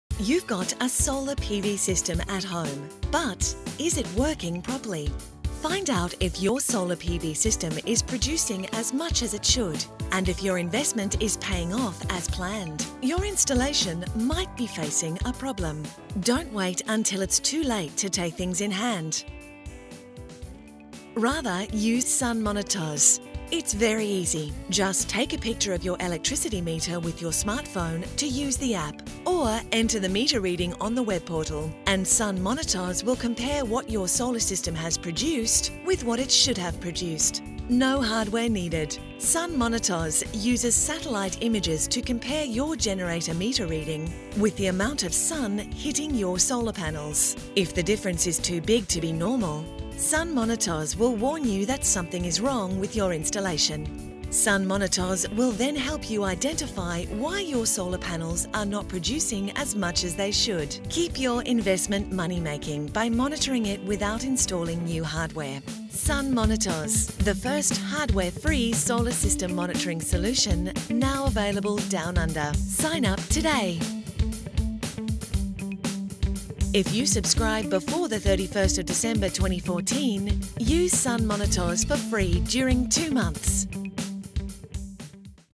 Sun MonitOZ Solar system monitoring
Sun MonitOZ is a solar system monitoring app that tells you if your solar panels are working properly.
Find out if your solar PV system is producing as much as it should and if your investment is paying off as planned.
Now available! Sign up on our website for more details